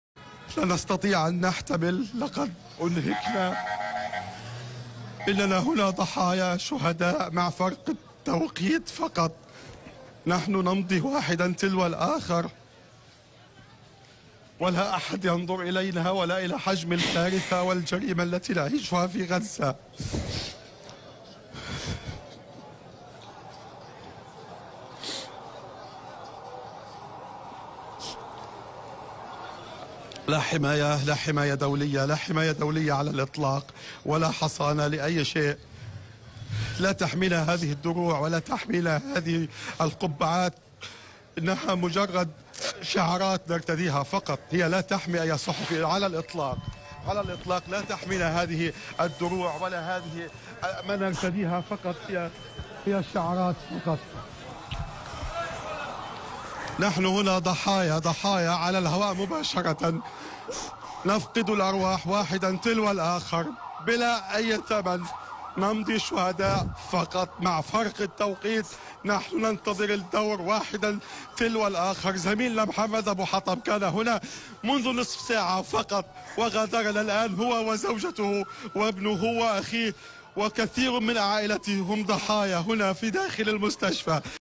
আমরা সবাই শহীদ, শুধু সময়ের ব্যবধান। এখানে কোনো অনাক্রম্যতা নেই, এখানে কোনো আন্তর্জাতিক আইন নেই!
সহকর্মী শহীদ হওয়ার পর গা/জা/নের সাংবাদিক তার সাংবাদিক ভেস্ট ও হেলমেট ছুড়ে ফেলে:
"আমরা সবাই শহীদ, শুধু সময়ের ব্যবধান। এখানে কোনো অনাক্রম্যতা নেই, এখানে কোনো আন্তর্জাতিক আইন নেই এবং দুর্যোগের মাত্রা নিয়ে কেউ চিন্তা করে না।"